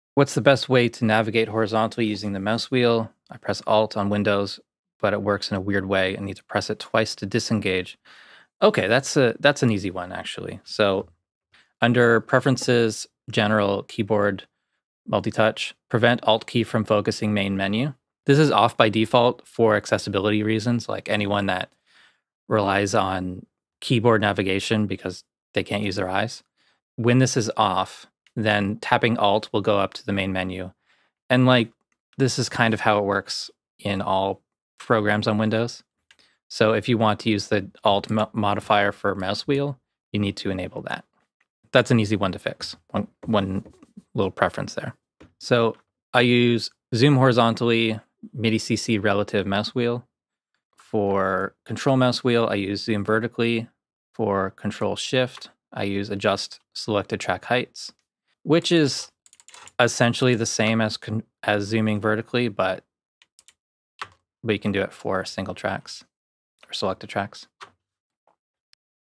Prevent ALT key from focusing menu in REAPER
This is a clip from a recent live stream answering a question about zooming with the mousewheel and preventing unwanted behavior with the alt key.